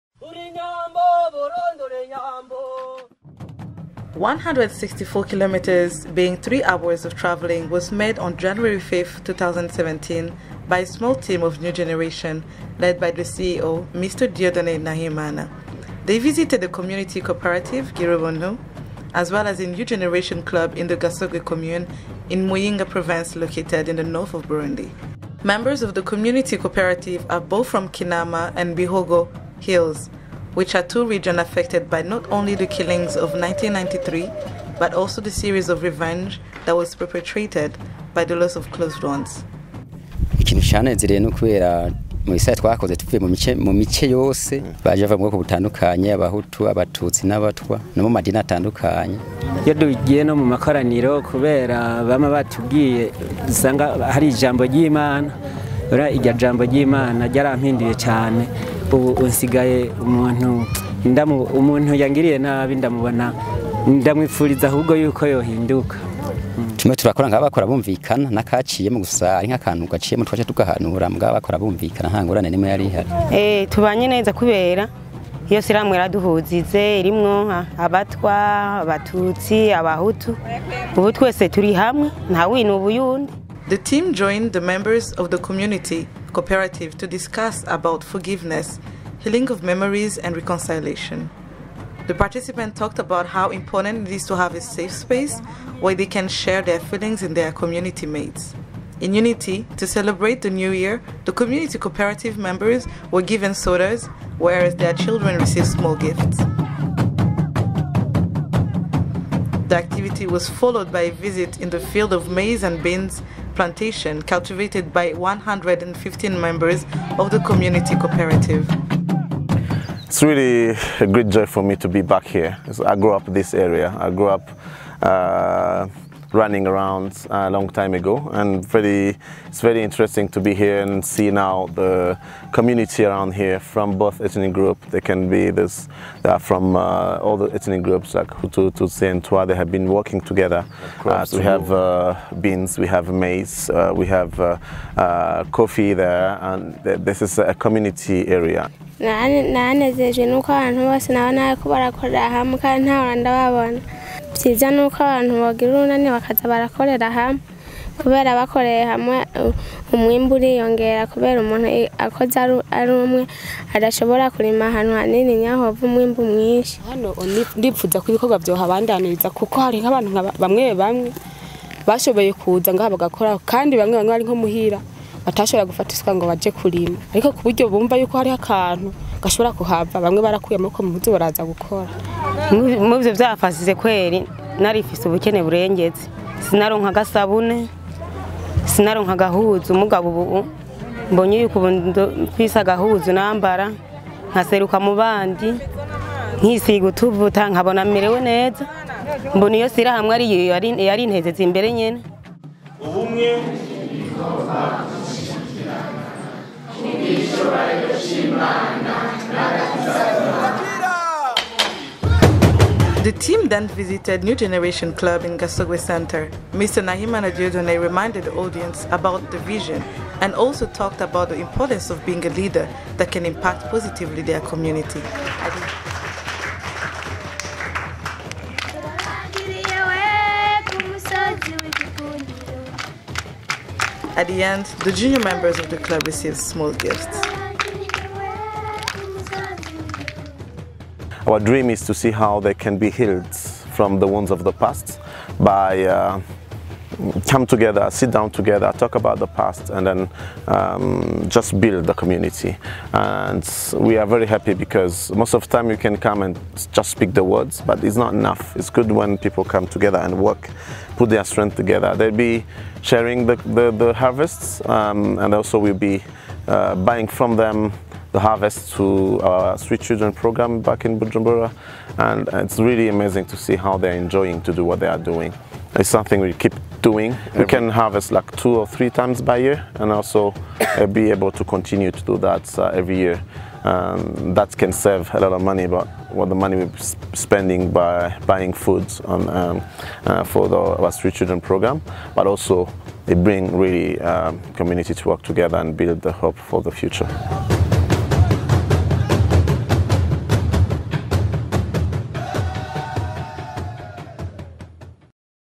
Dieudonne Nahimana (New Generation Burundi) visited Gasorwe
On January 5th, 2017, a small team of New Generation led by Dieudonné NAHIMANA CEO, and Teddy Claude HARUSHIMANA CPO, visited a Community Cooperative « Gir'Ubuntu » as well as a New generation club in the Gasorwe Commune, Muyinga province, in the North of Burundi.